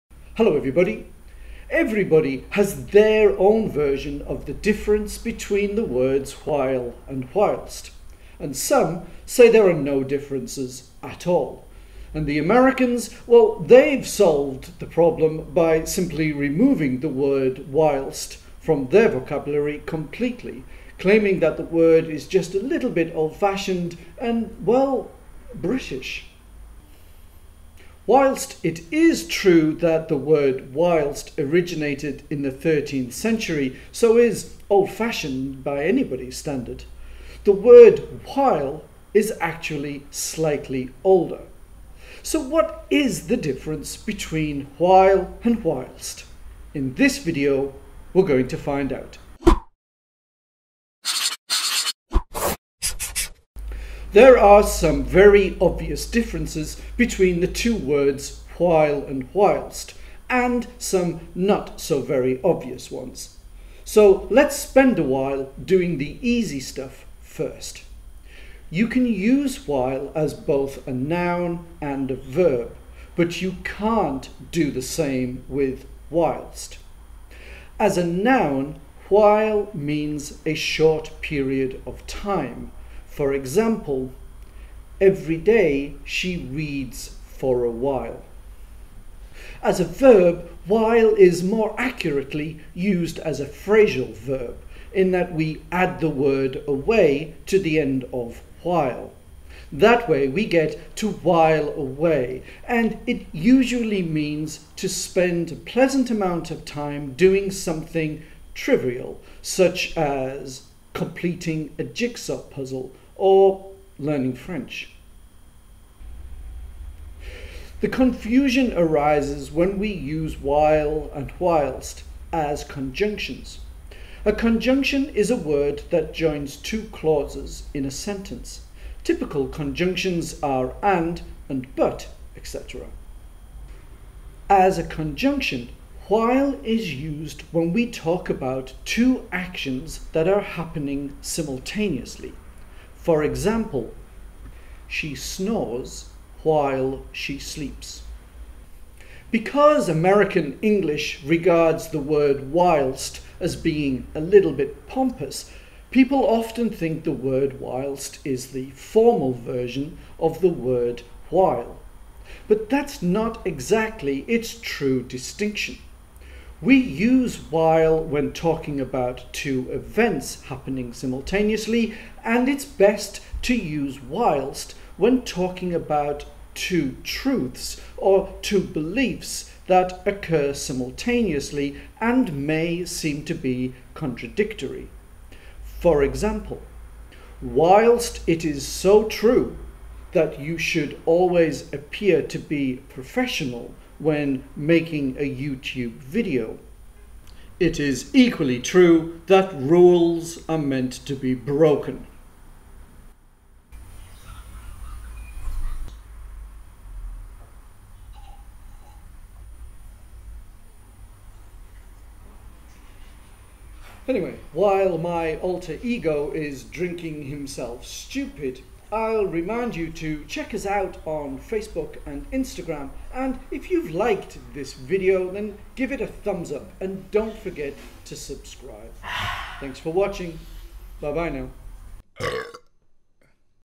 While Vs Whilst
The difference between "while" and "whilst" in English.